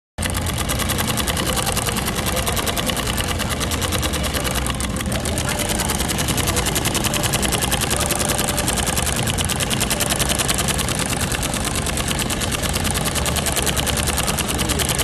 Chainstitch Embroidery
Hand cranked chainstitch on the spot at The Harder They Fall Netflix movie premier event in NYC!